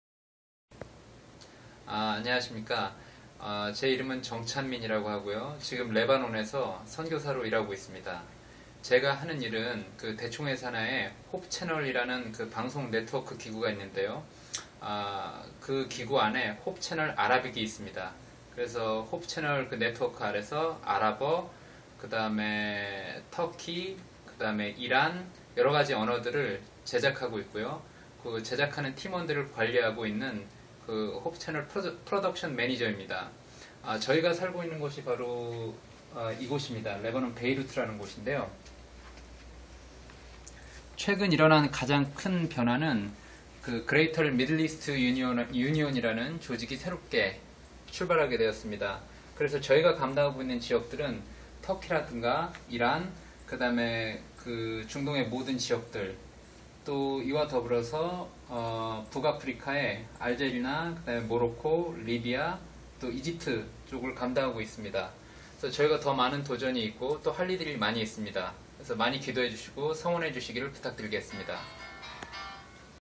[권정행] 레바논에서 일하는 정찬민 목사
정찬민 목사는 레바논에서 미디어 사역을 하고 있다